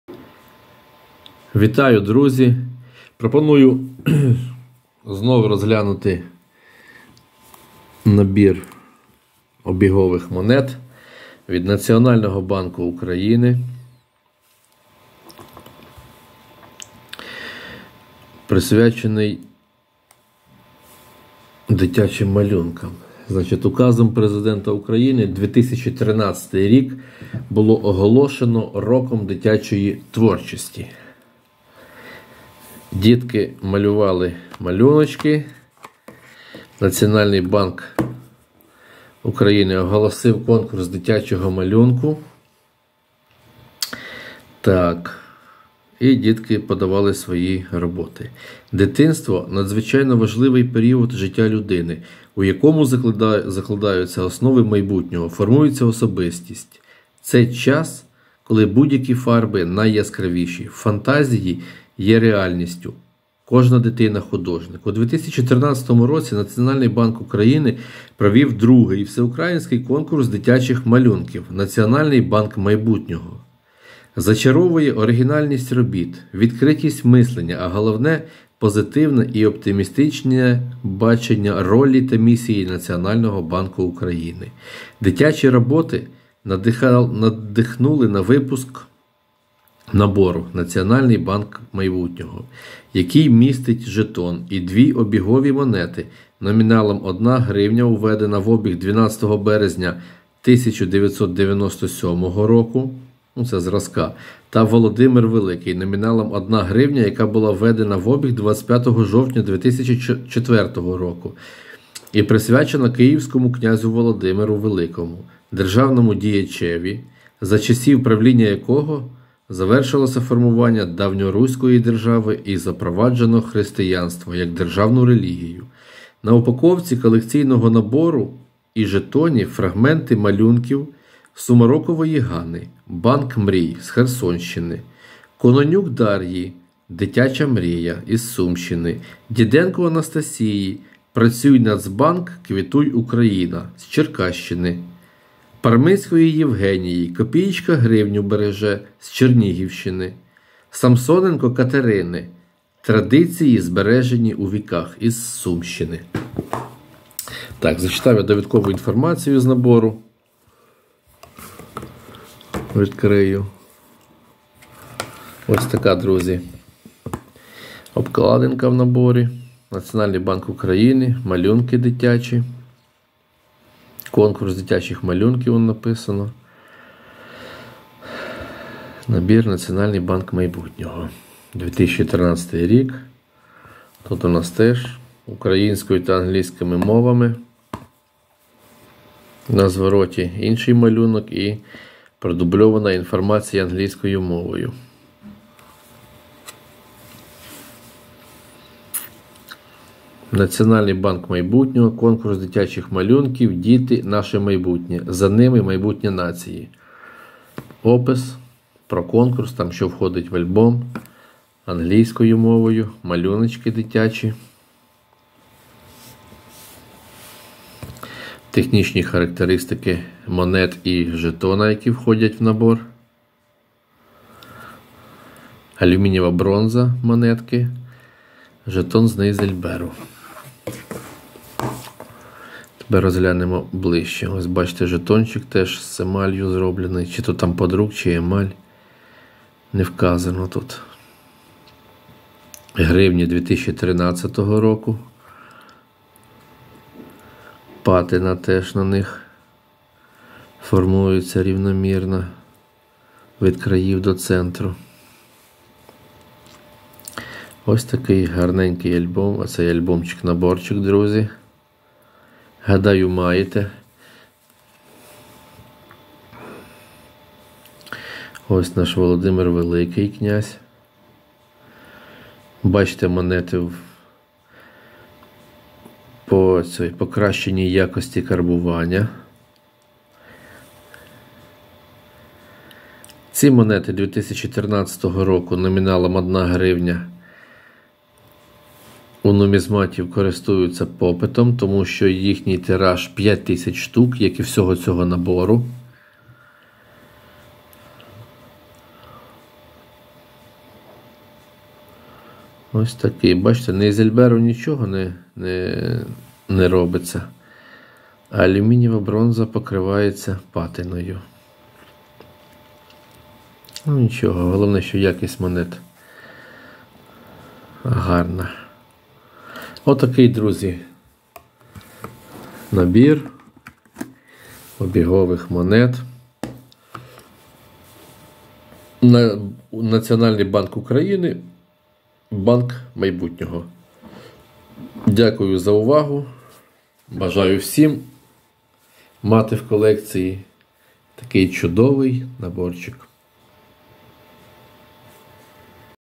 Колекційний набір Монети України "Національний банк майбутнього" 2013. Coins of Ukraine NBU Mint.
Колекційний набір Монети України "Національний банк майбутнього" 2013. Collection set of Coins of Ukraine "National Bank of the Future" 2013.

Указом Президента України 2013-й було оголошено Роком дитячої творчості. За доброю традицією цього року Національний банк провів уже ІІ Всеукраїнський конкурс дитячих малюнків “Національний банк майбутнього”, мета якого – підвищення рівня знань дітей про центральний банк країни, його функції та завдання, а також виявлення творчих здібностей дітей.

Технічні параметри обігових монет, з яких сформовано набір, не відрізняються від технічних параметрів цих монет попередніх років карбування, крім зазначеного на аверсі року, – 2013.

На упаковці колекційного набору і жетоні – фрагменти малюнків Ганни Сумарокової “Банк мрій” (з Херсонщини), Дар’ї Кононюк “Дитяча мрія” (із Сумщини), Анастасії Діденко “Працюй, Нацбанк - квітуй Україно" (з Черкащини),  Парминської Євгенії "Копієчка гривню береже" (з Чернігівщини), Самсоненко Катерини "Традиції, збережені у віках" (із Сумщини).

У набір увійшли випущені в покращеній якості розмінні монети усіх номіналів: 1 гривня 2013 року звичайною карбування, 1 гривня 2013 року «Володимир Великий», пам'ятний жетон «Добро дітям»

Тираж - 5 000 штук.

By decree of the President of Ukraine, 2013 was declared the Year of Children's Creativity. Following a good tradition, this year the National Bank has already held the 2nd All-Ukrainian children's drawing competition "National Bank of the Future", the purpose of which is to increase the level of children's knowledge about the country's central bank, its functions and tasks, as well as to reveal children's creative abilities.

The technical parameters of the circulating coins, from which the set was formed, do not differ from the technical parameters of these coins of previous minting years, except for the year indicated on the obverse, which is 2013.

On the packaging of the collection set and the token - fragments of drawings by Anna Sumarokova "Bank of Dreams" (from Kherson Region), Daria Kononyuk "Child's Dream" (from Sumy Region), Anastasia Didenko "Work, National Bank - Bloom Ukraine" (from Cherkasy Region), Evgenia Parmyna "A coin saves a hryvnia" (from Chernihiv Oblast), Kateryna Samsonenko "Traditions Preserved in the Ages" (from Sumy Oblast).

The set includes exchange coins issued in improved quality of all denominations: 1 hryvnia of 2013 with regular minting, 1 hryvnia of 2013 "Vladimir the Great", commemorative token "Good for Children"

Circulation - 5,000 pieces.